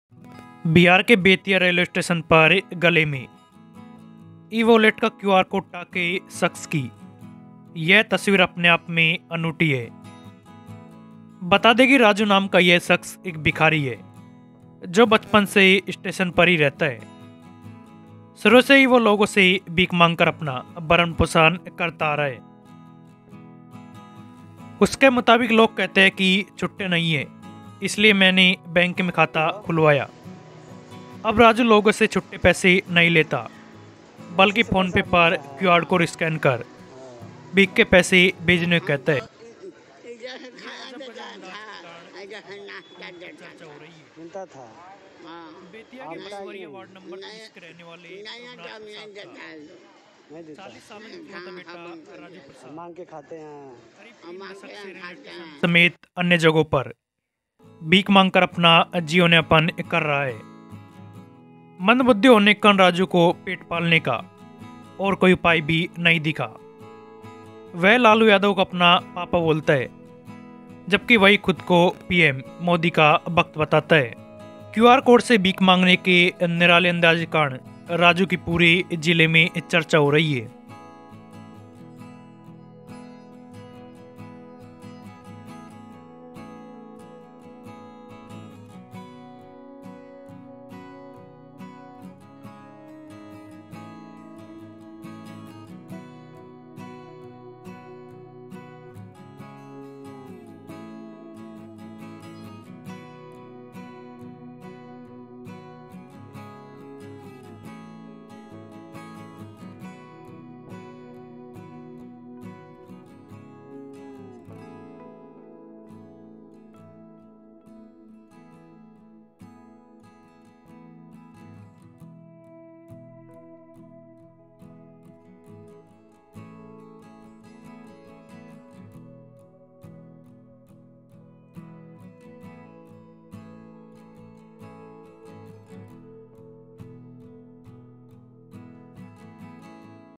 डिजिटल की इस दुनिया में भिखारी भी डिजिटल, लोग बनाने थे खुल्ले पैसे का बहाना तो गले में लगाया QR कोड
डिजिटल की इस दुनिया में भिखारी भी डिजिटल, लोग बनाने थे खुल्ले पैसे का बहाना तो गले में लगाया QR कोड स्कैन
भिखारी का नाम राजु प्रसाद है जो बिहार के बेतवा रेलवे स्टेशन पर भीख मांगकर अपना गुजारा करता है